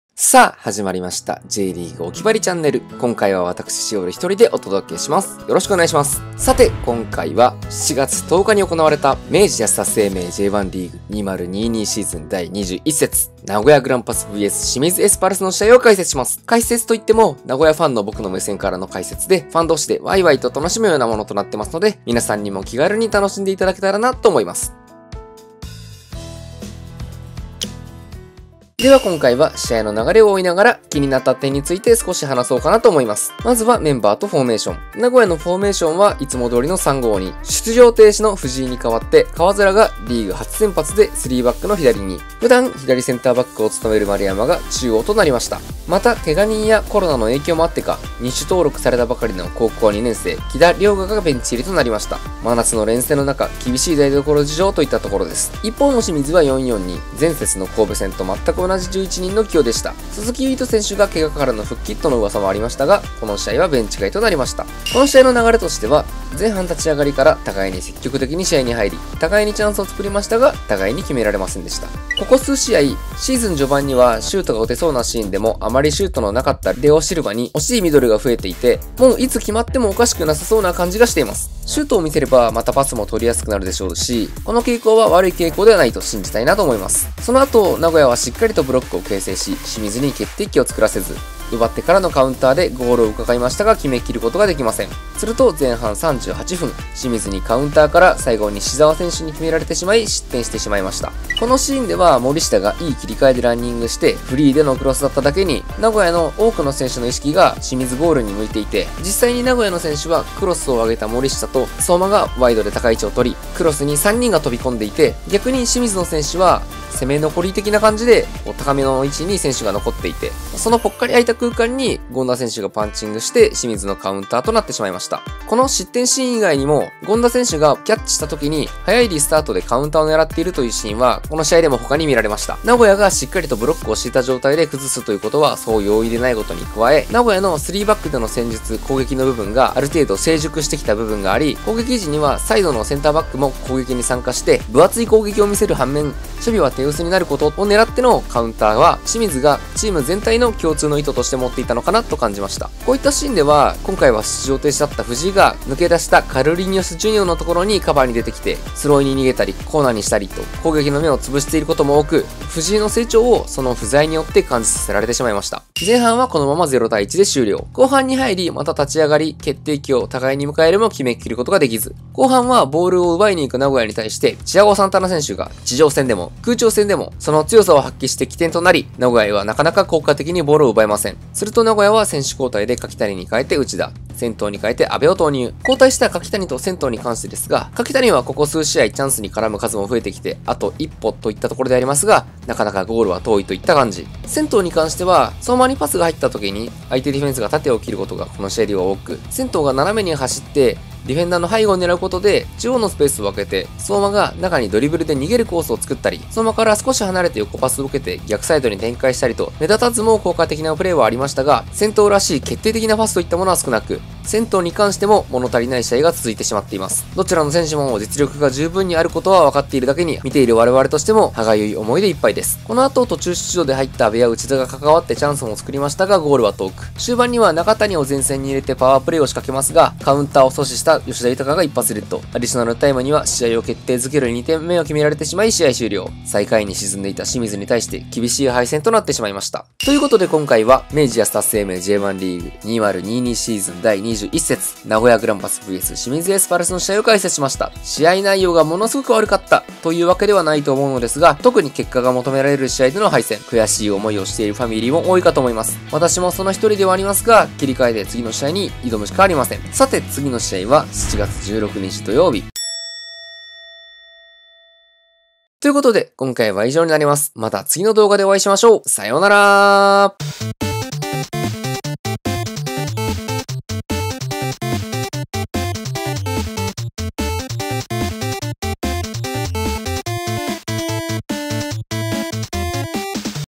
【コロナ・怪我人・真夏の連戦】前回対戦同様狙い通りのロングカウンターから失点・・・明治安田生命J1リーグ第21節名古屋グランパスvs清水エスパルス解説【相馬・中谷は代表で成長してきてくれ】
どうもこんにちは。Jリーグおきばりチャンネルのシオルです。
幸か不幸か早めの中断期間へ
新戦力を迎え入れ巻き返しに期待
(シオル)